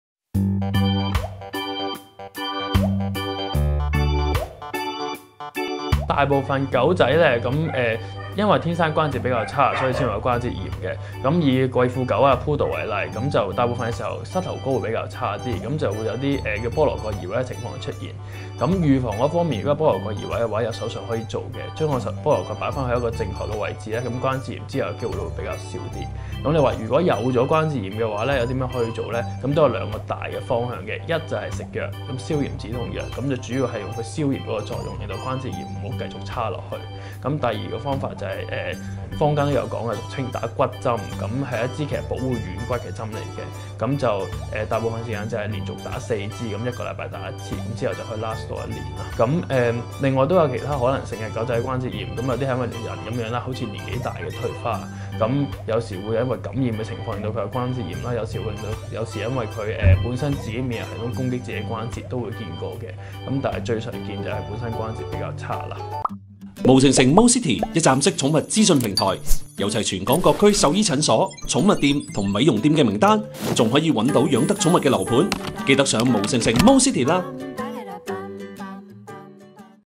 【寵物講醫D】狗狗關節炎的成因與治療
【小心關節炎】近日天氣轉涼，很多狗狗會變得唔想郁，或者動作僵硬，咁樣可能是關節炎發作。

香港愛護動物協會註冊獸醫鄭德聖表示，很多狗狗如貴婦狗，關節比較差，易出現膝蓋骨移位，而引發關節炎。另一常見的病發原因，是因為狗狗年紀漸大關節出現退化，導致關節炎。

如果狗狗有關節炎， 鄭德聖獸醫表示有兩個常見的治療方法，第一是服用消炎止痛藥，消炎令關節炎不會再惡化下去。第二個方法是注射保護軟骨的針，即是坊間俗稱的「打骨針」，多數是連續打四支，每一個星期打一支，若可以維持一年。

一站式寵物資訊平台-【毛城城MoCity】，想睇更多有關寵物嘅猛料? 記得上 喇！